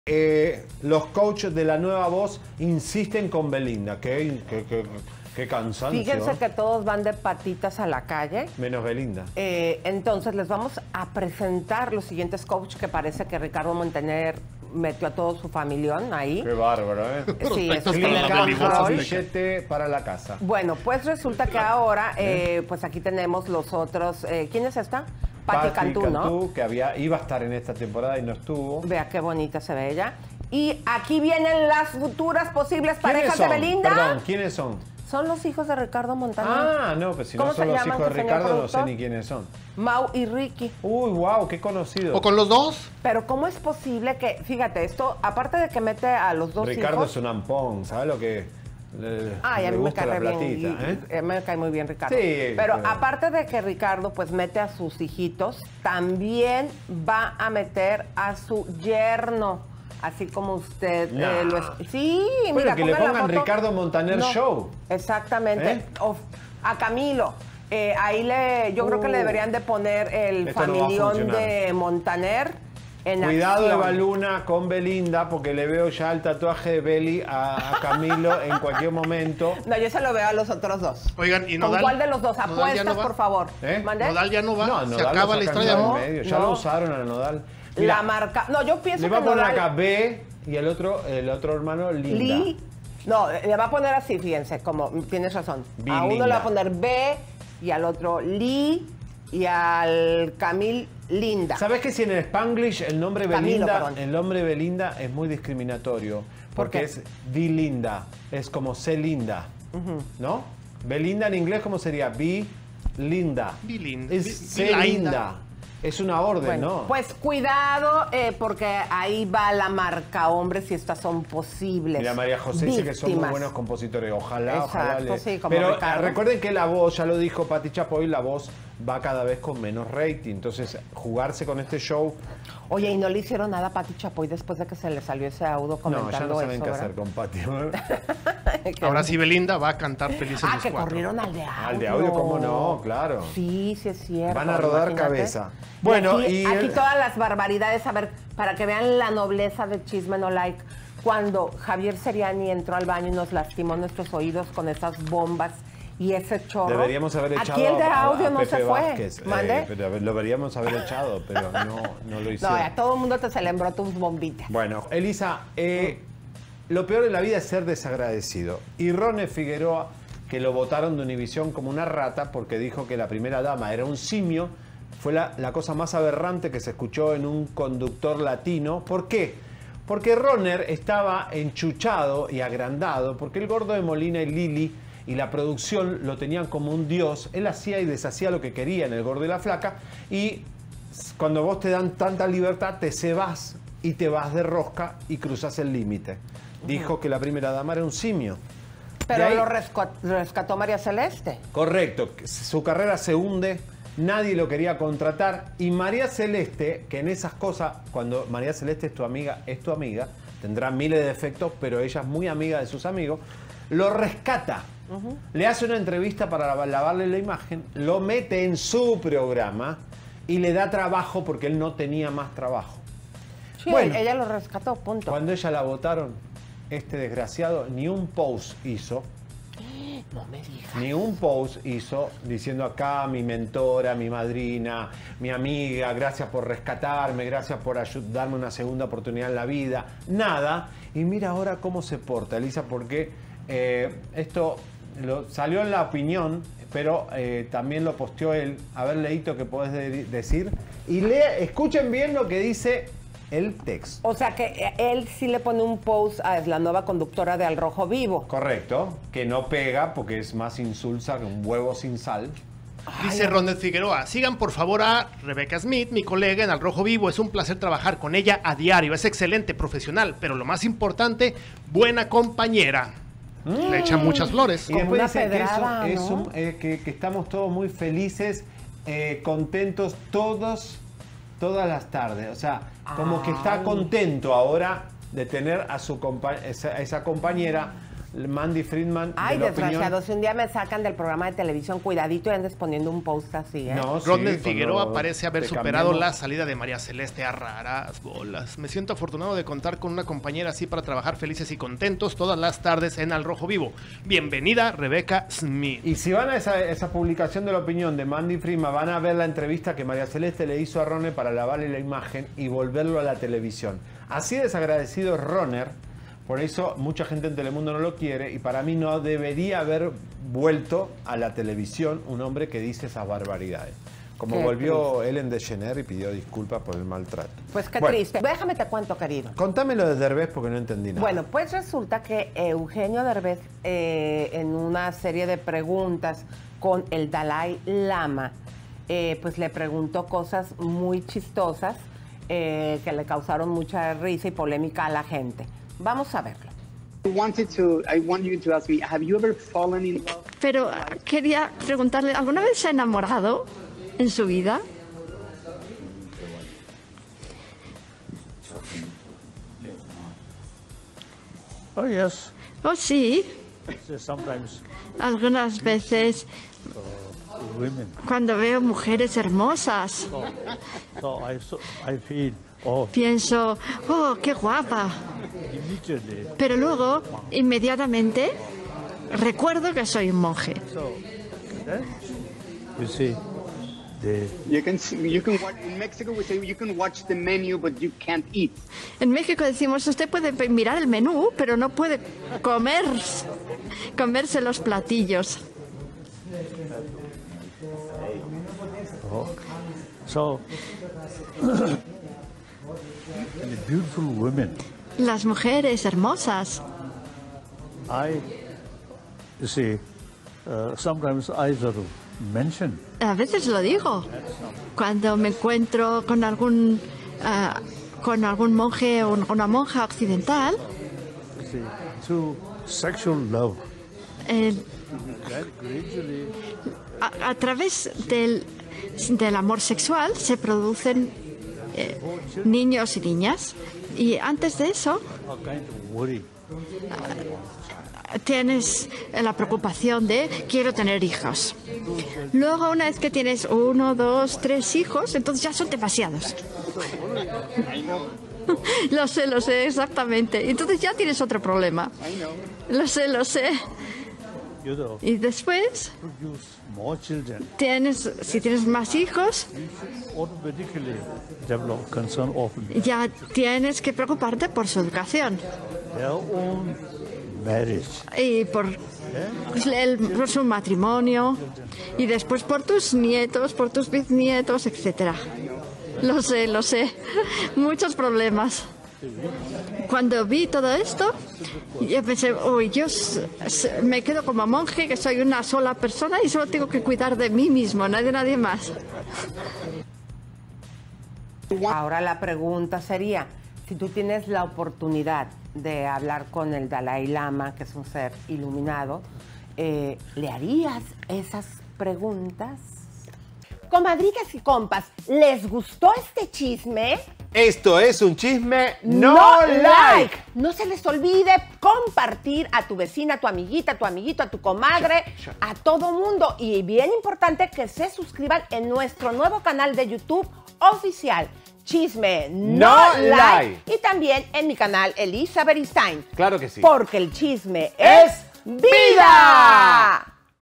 BELINDA SE QUEDA SIN NODAL, RICARDO MONTANER ACOMODA A SU GENTE EN LA VOZ!- CHISME NO LIKE
Cristian Nodal queda fuera de la Voz para dar paso a la familia de Ricardo Montaner, la que se queda es Belinda, esperemos que no marque a otro más...